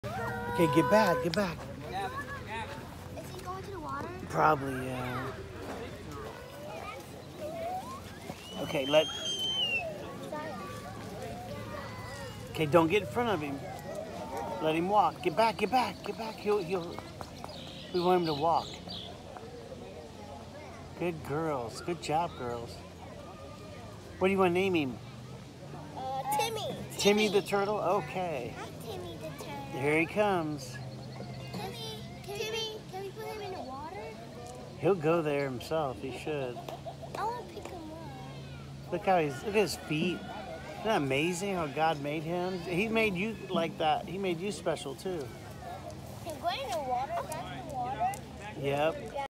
Turtle found in Armstrong ball field
He wanted to learn how to play baseball with the kiddos